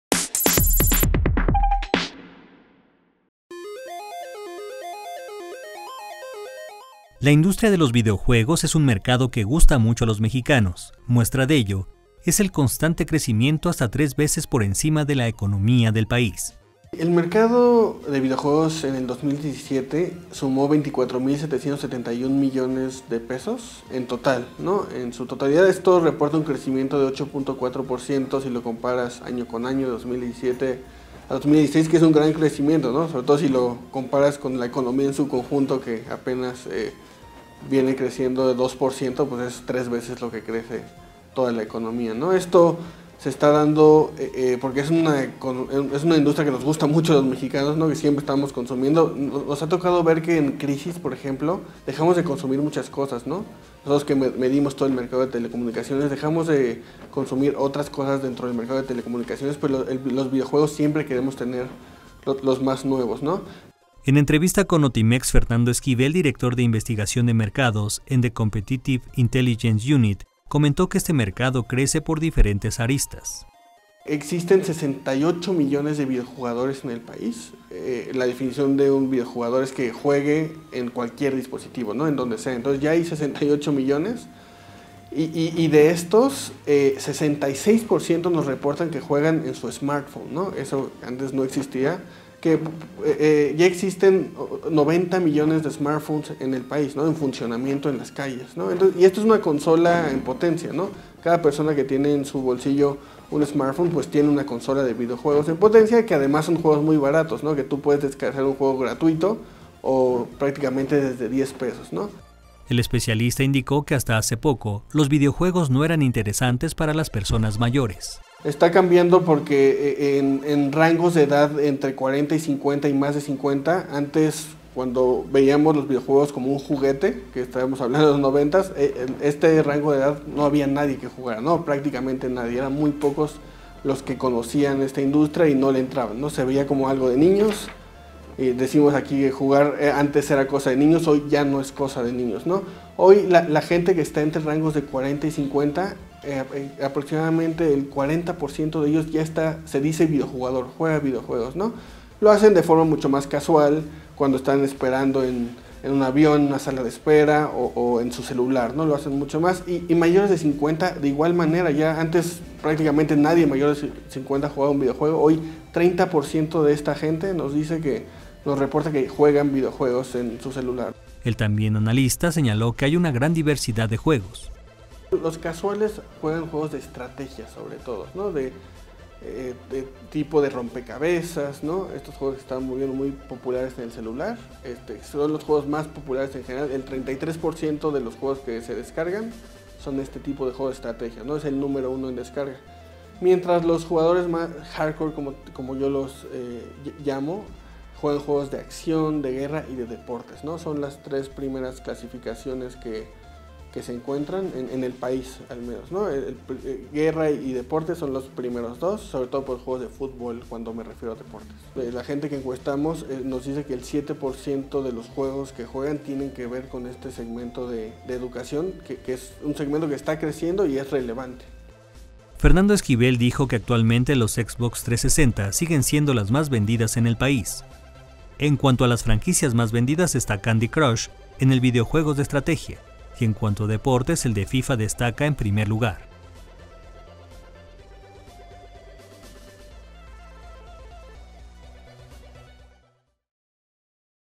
La industria de los videojuegos crece más que la economía nacional
Existen 68 millones de videojugadores en México; el mercado sumó 24 mil 771 millones de pesos durante 2017